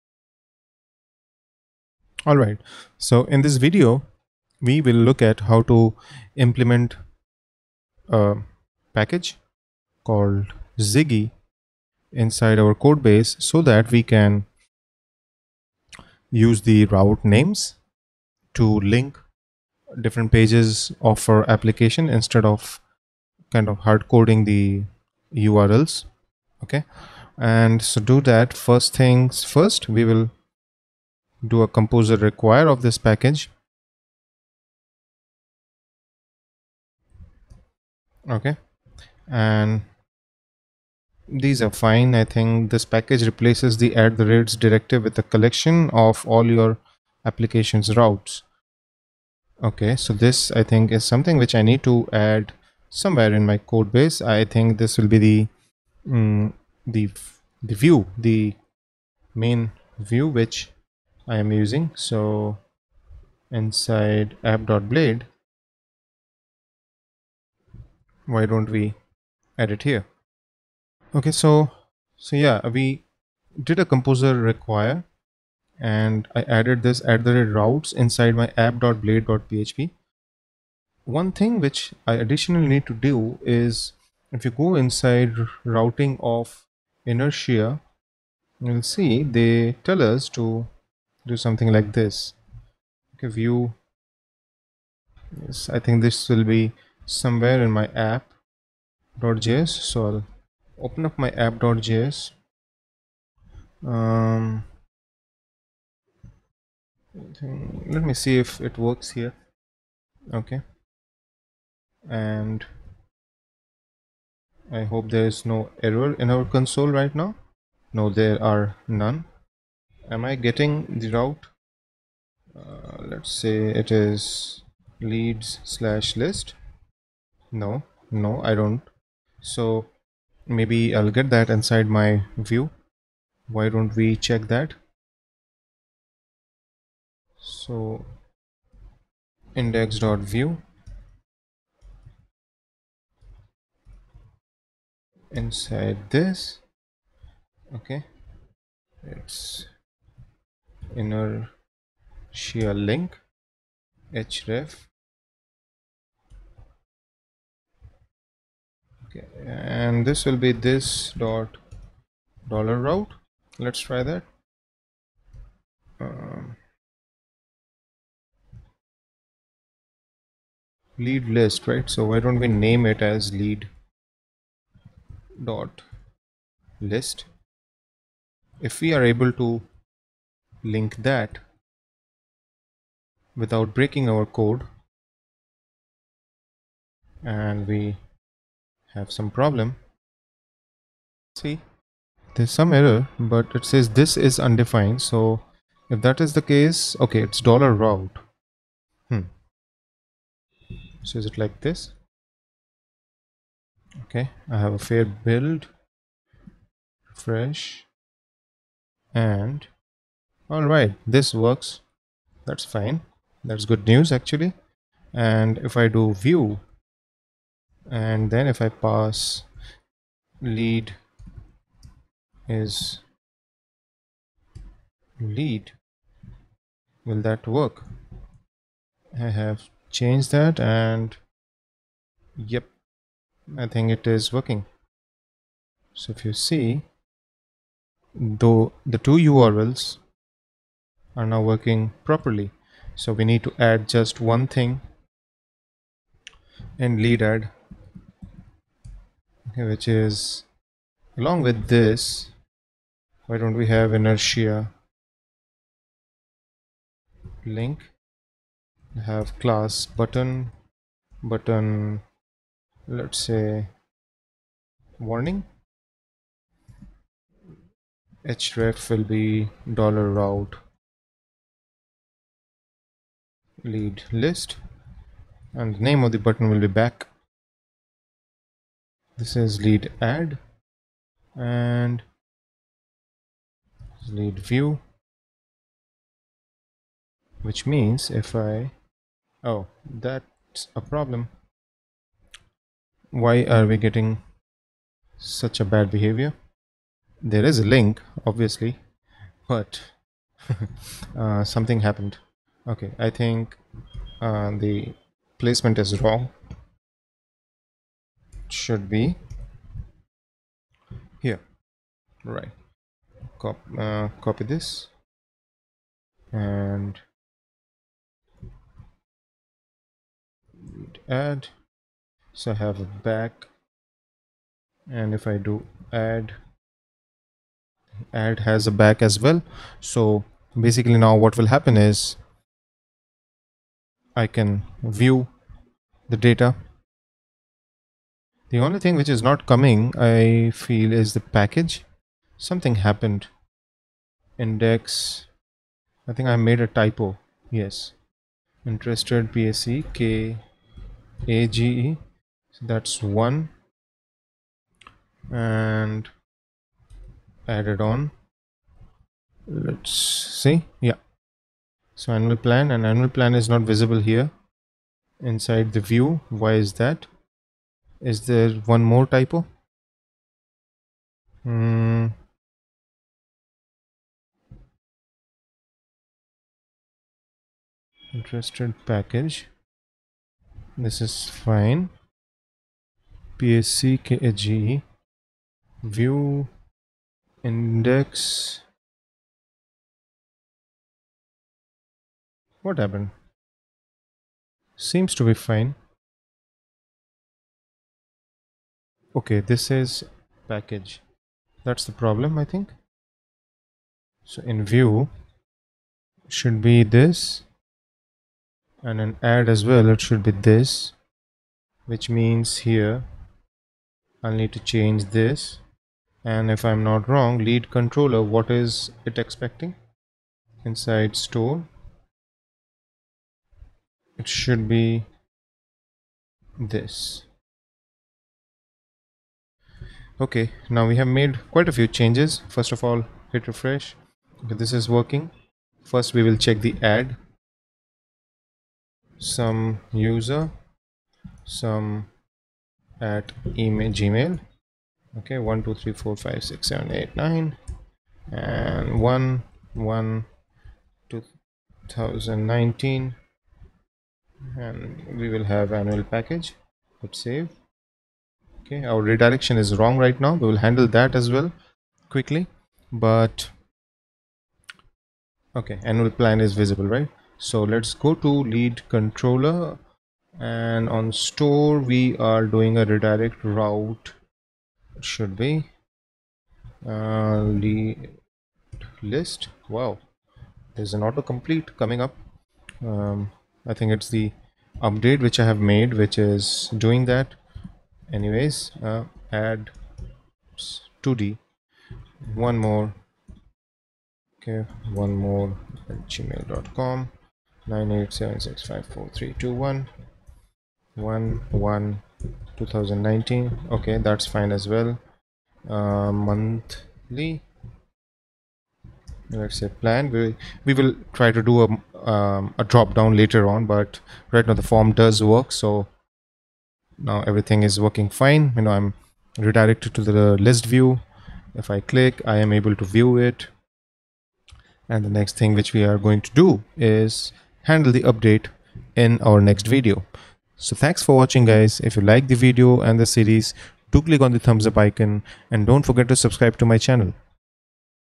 09 - Using the ziggy library with InertiaJS for handling routes
Ziggy package which will allow us to work with named routes inside our javascript application. In this video, we are going to see how we can use ziggy package. Instead of maintaining URLs inside our codebase and then keeping a track and manually changing them when we do a refactor, this powerful package allows us to just use the routes as if we are inside the Laravel codebase and use almost all of the routes like features inside Javascript.